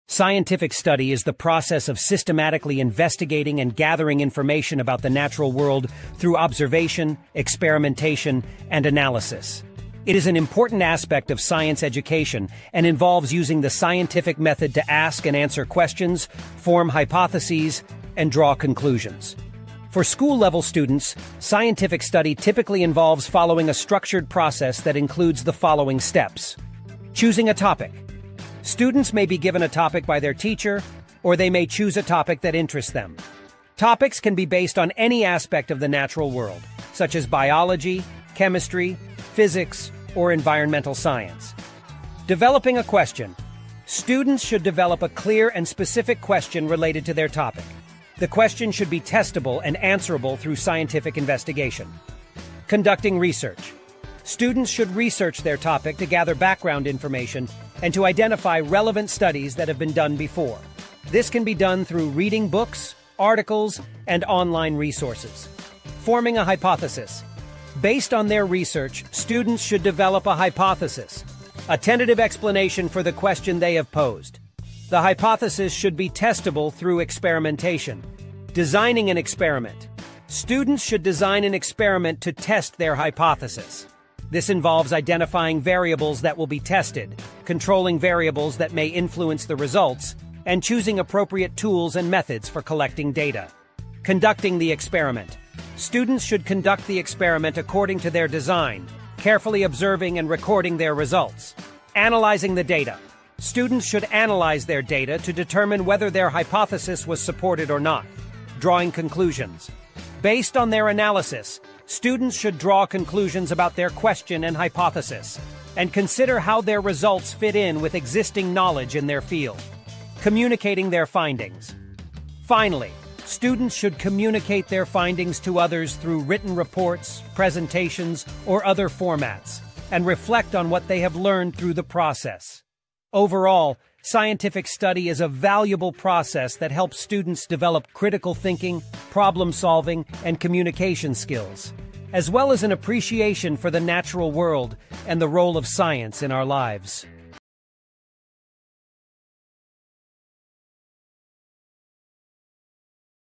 Scientificstudyis Class 10
"Are you interested in science and want to learn more about scientific study? In this video, we'll explore the process of scientific study and how you can use it to investigate the natural world. Whether you're a student or just curious about science, this video will guide you through the steps of scientific study, from choosing a topic to communicating your findings. You'll learn about the scientific method, how to develop a hypothesis, design an experiment, and analyze data, as well as the importance of communication and reflection in the scientific process. So, join us for an exciting journey into the world of scientific study, and discover how you can become a scientist in your own right! Let's get started!"